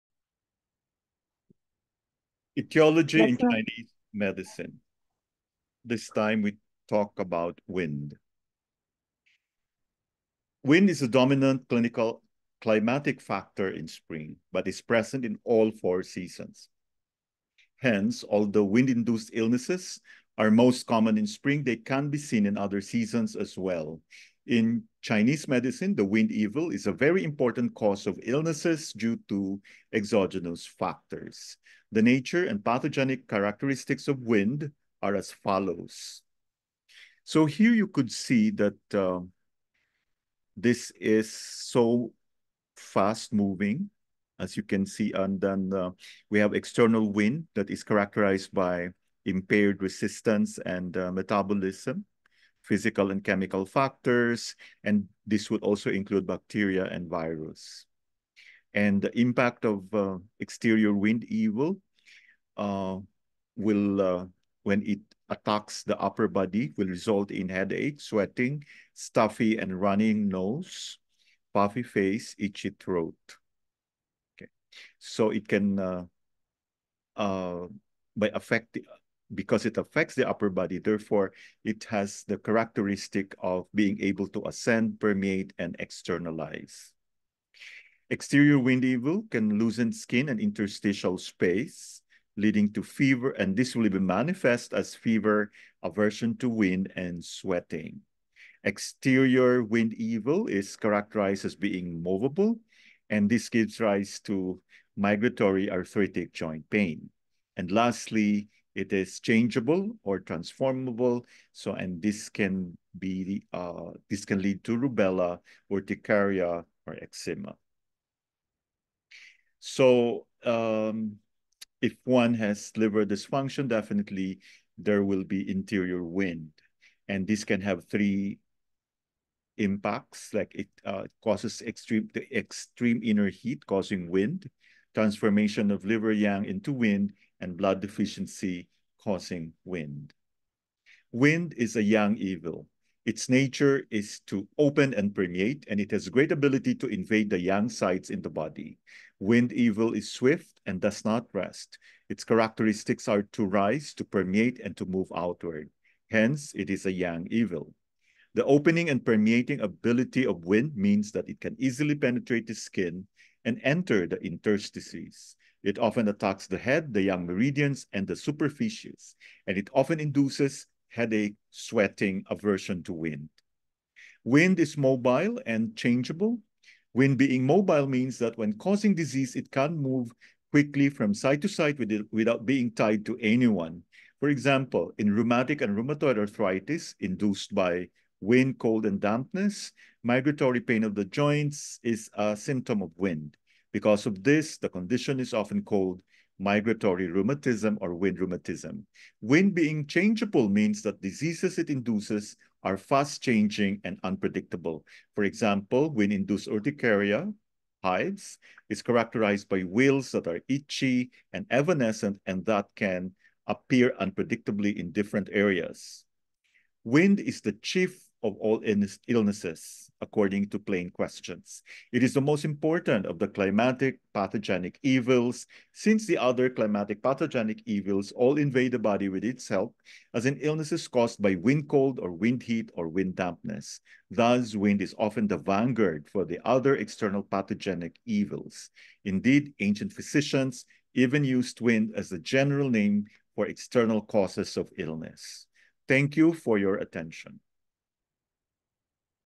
NATURE AND PATHOGENIC CHARACTERISTICS OF WIND
Wind is one of the Six Pernicious Influences. As a pathogenic factor, it is seen as a major external cause of diseases. Wind is Yang in nature and tends to injure Blood and Yin. Wind is often the vehicle through which other climatic factors invade the body. For example, Cold will often enter the body as Wind Cold and Heat as Wind Heat.